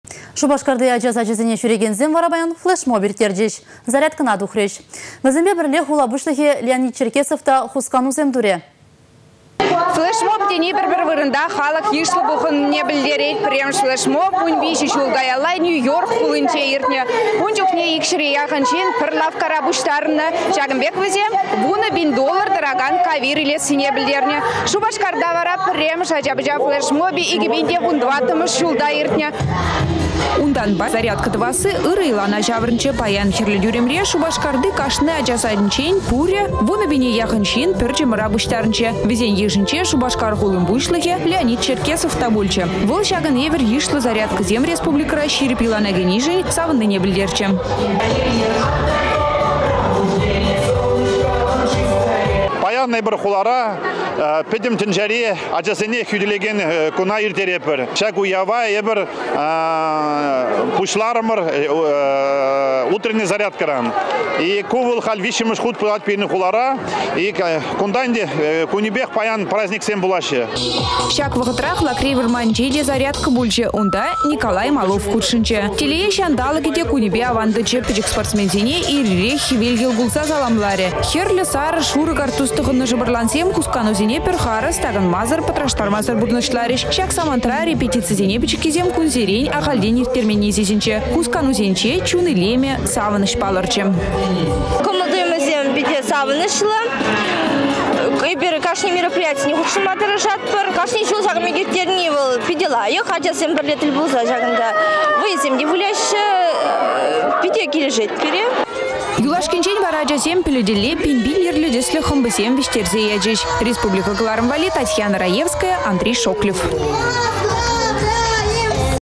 10 пин ача зарядкăна тухрĕ
Шупашкарти ача сачĕсене çÿрекенсем вара паян флешмоб ирттерчĕç, зарядкăна тухрĕç. Вĕсемпе пĕрле хула пуçлăхĕ Леонид Черкесов та хусканусем турĕ.